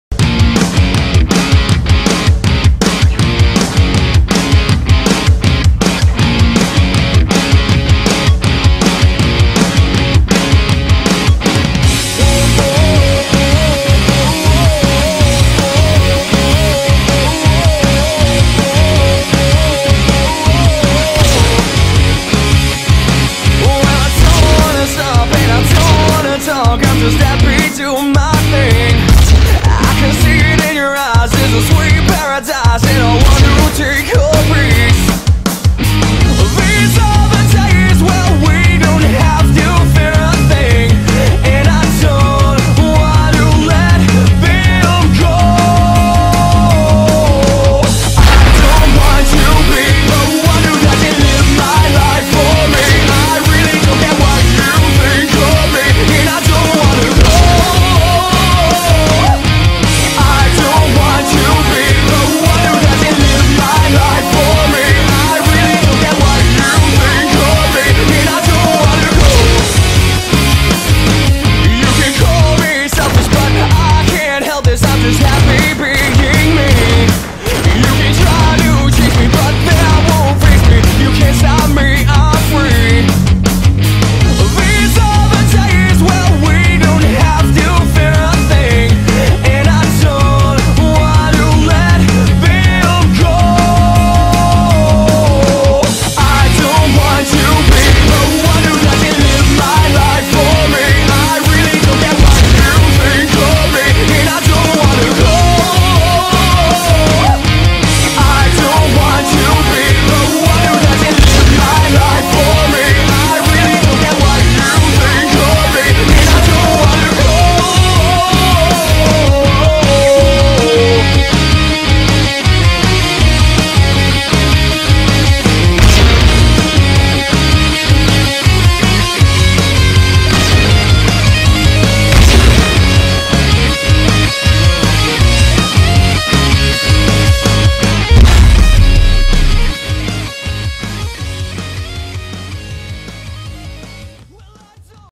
SoaR uNQ: "Watch My Steeze" - Episode 6
Some amazing shots and feeds from the amazing uNQ! Also, I can't credit the editor enough for the effort he put into this video, the cinematics were just beautiful! Be sure to check their channels out :)
- Ollie

Like our brand new Facebook page!
1000 likes = Livestream & Open Lobby!